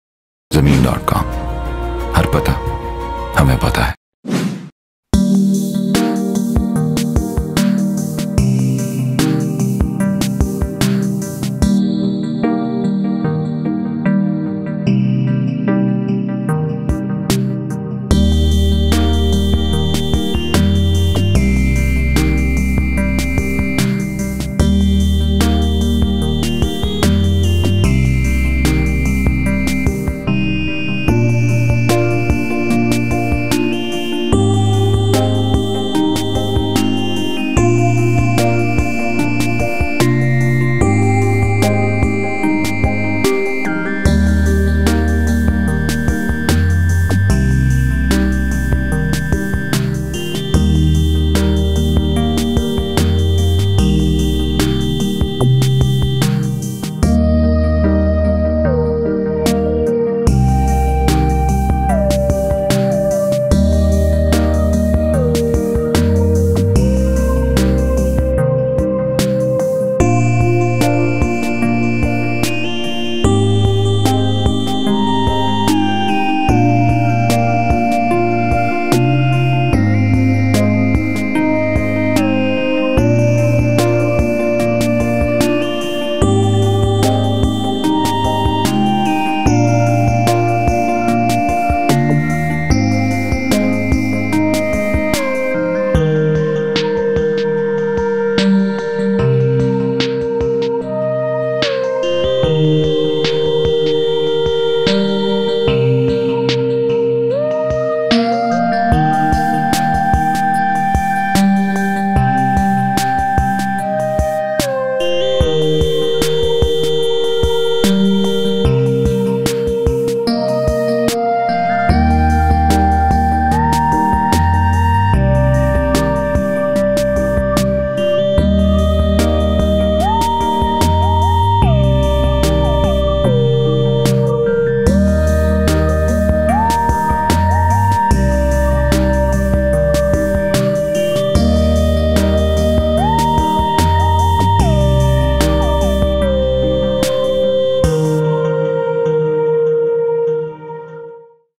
2.7 KANAL UPPER PORTION FOR SALE IN AZAM TOWN KARACHI
3rd Floor Tile Flooring American Kitchen Brand New Luxury Apartment.
Having A Home Of Your Choice Is The Ultimate Blessing. Well, That Blessing May Be Yours Sooner Rather Than Later, Because
And also more option available.